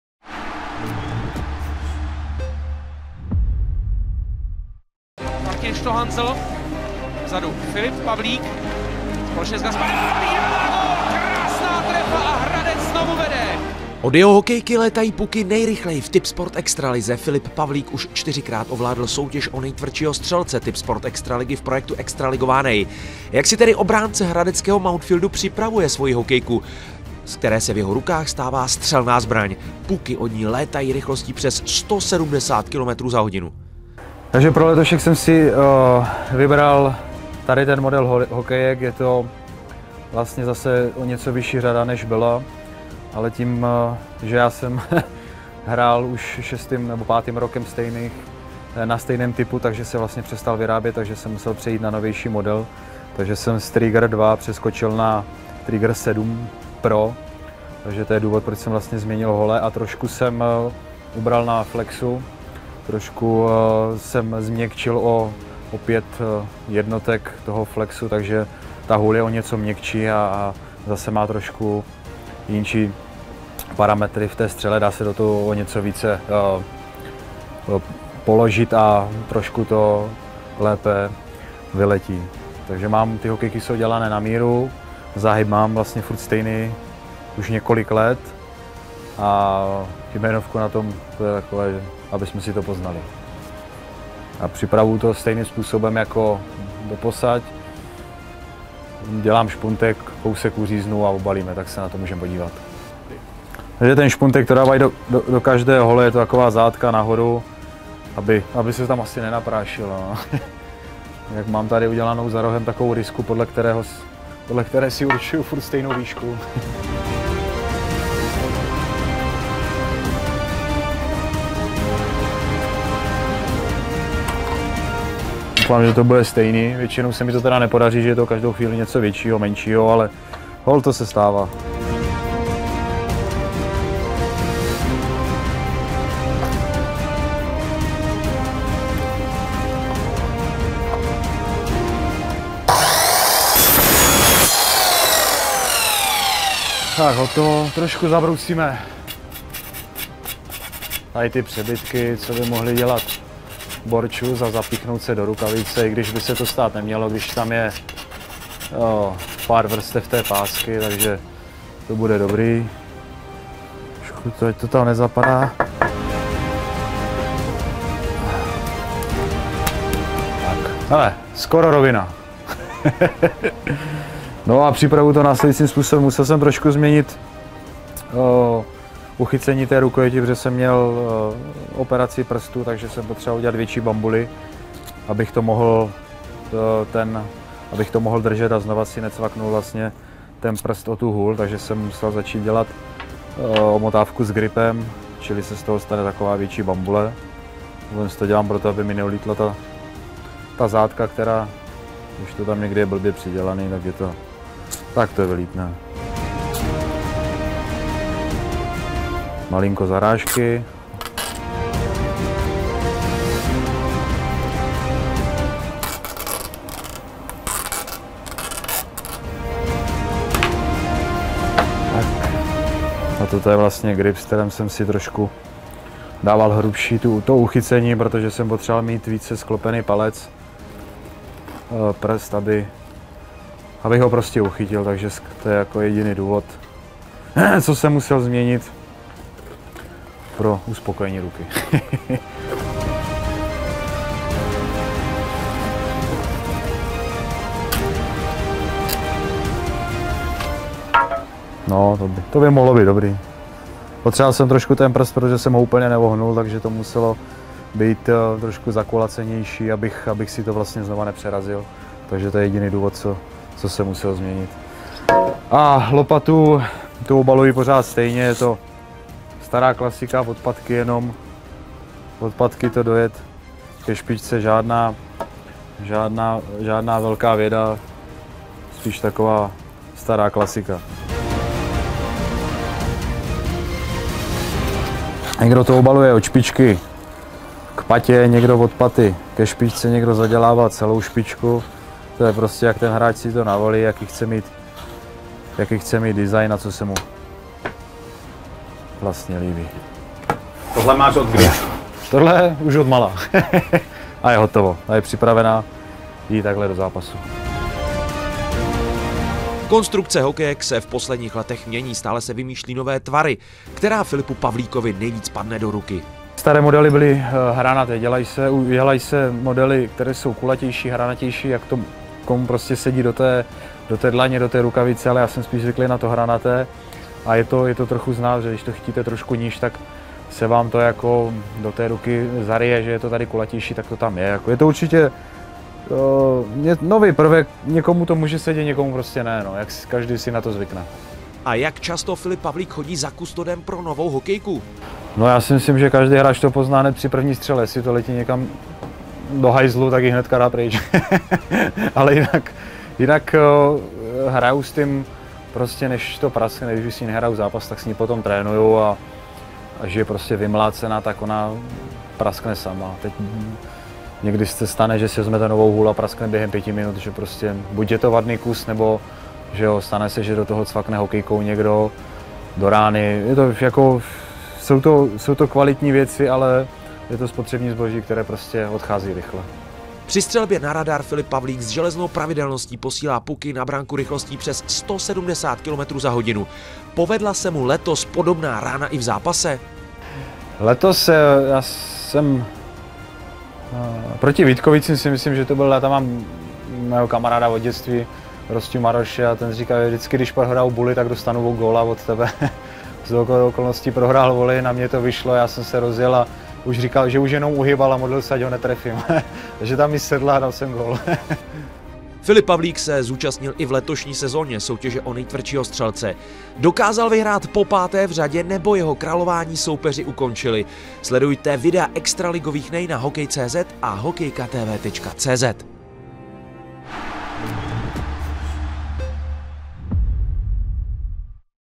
Ránu má největší. Jak upravuje hokejku nejtvrdší střelec extraligy?
Do ruky dobře padnoucí hokejka je jedním z předpokladů přesné i tvrdé střely. Jak si tedy svoji hůl upravuje nejtvrdší střelec Tipsport extraligy? S kamerou vás zavedeme přímo do kamrlíku kustoda hradeckého Mountfieldu, kde si hokejku k obrazu svému upravuje Filip Pavlík. Jak jí zkracuje, jak jí omotává, proč na ní musí v poslední době dělat bambuli? Obránce Mountfieldu HK vám také prozradí, jak často hokejky mění. Víte, kdy se mu letos v zápase povedla dělovka, která láme kosti?